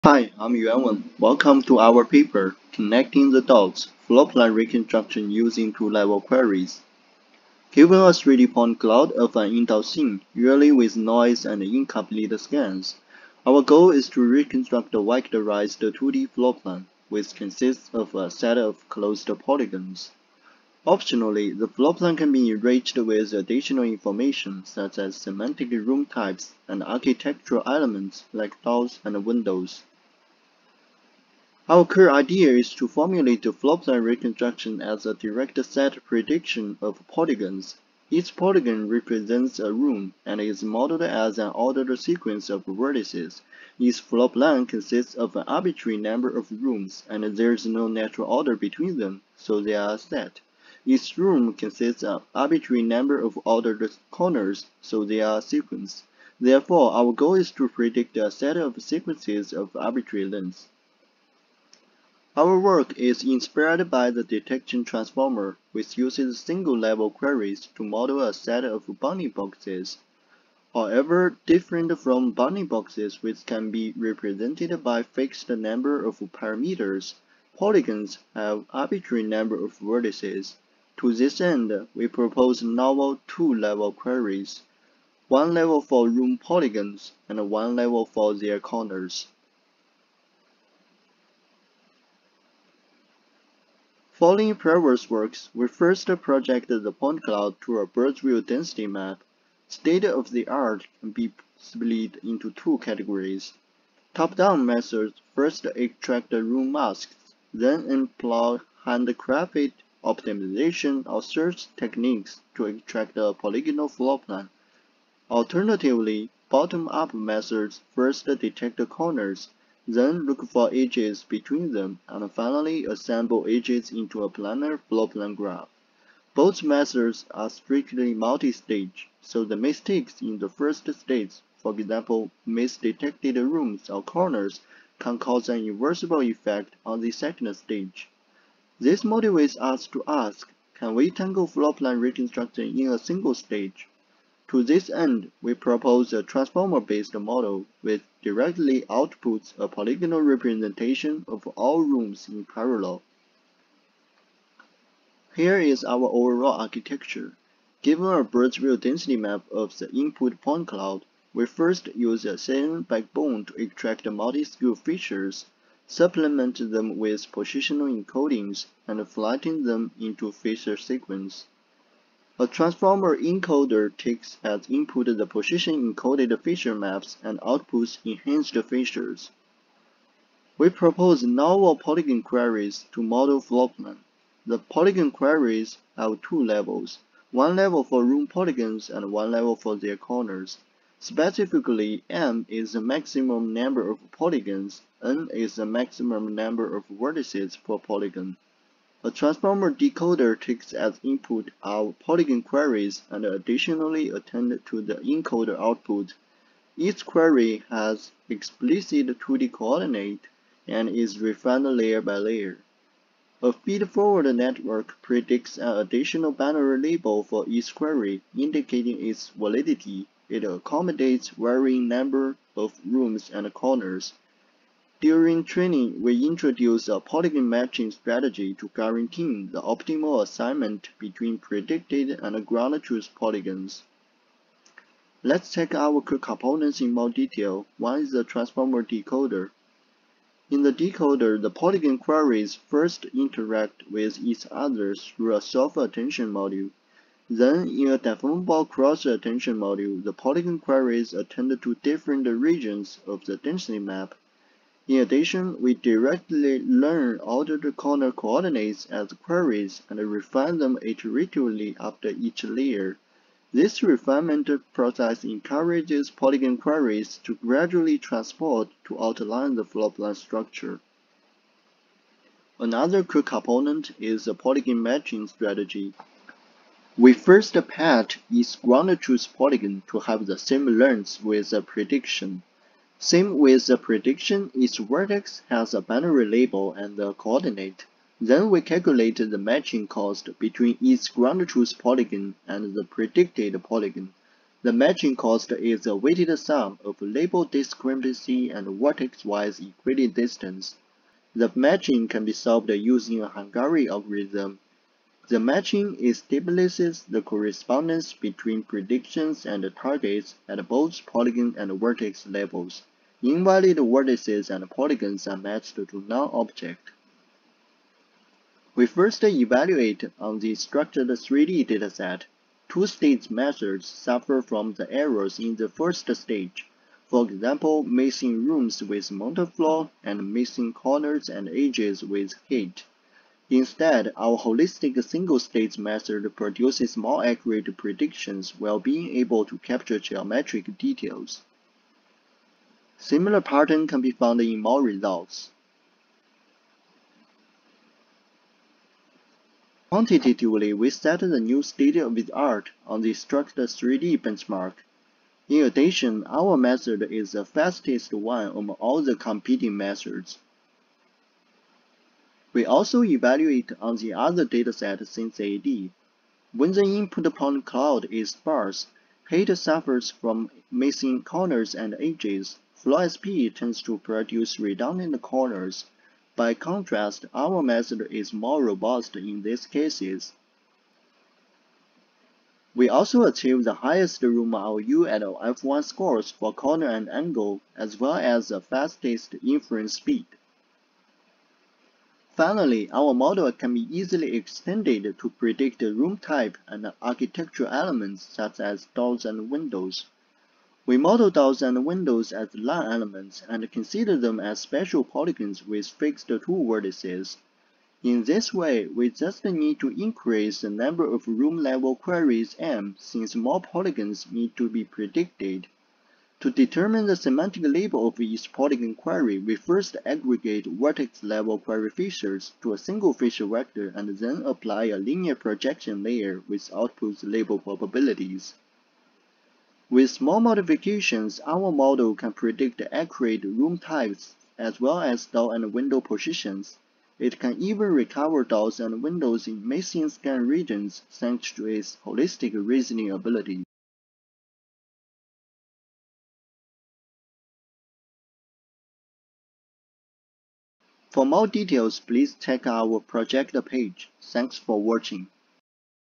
[CVPR'23] Connecting the Dots: Floorplan Reconstruction Using Two-Level Queries
We present RoomFormer: two-level queries for single-stage floorplan reconstruction.